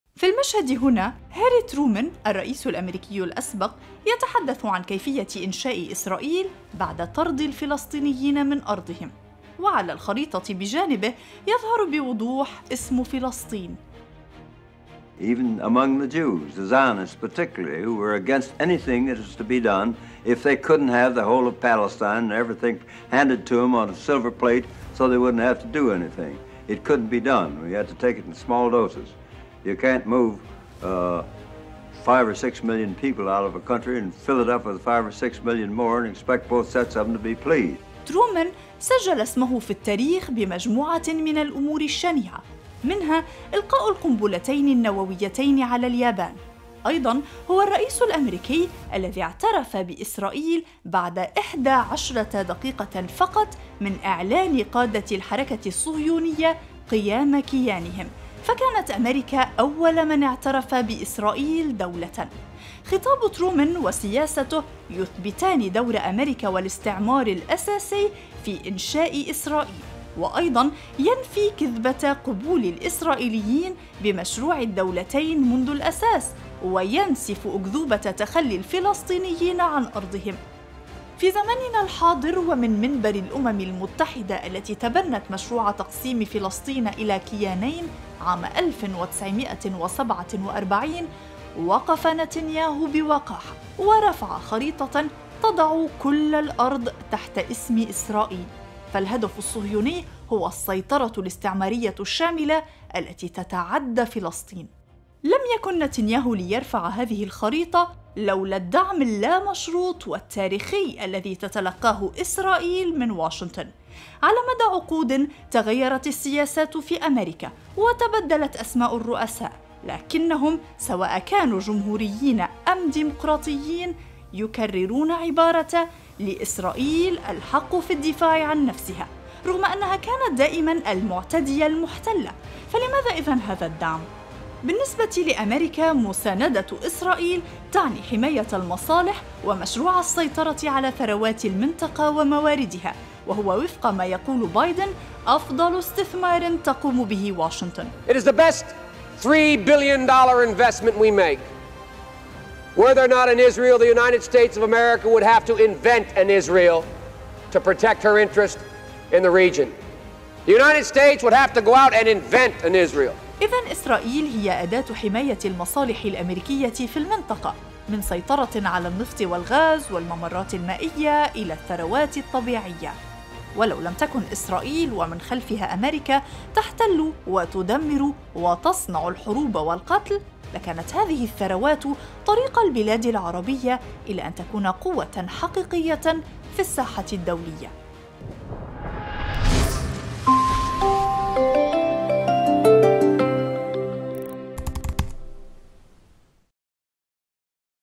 منذ ترومان إلى بايدن.. تحمي أميركا "إسرائيل" لأنها جزء من مصالحها
إسرائيل" بالنسبة إلى أميركا ليست "دولة" حليفة بل هي قاعدة استعمارية في مشروع السيطرة على الثروات
إعداد: سارة خازم
تنفيذ: محمد خلف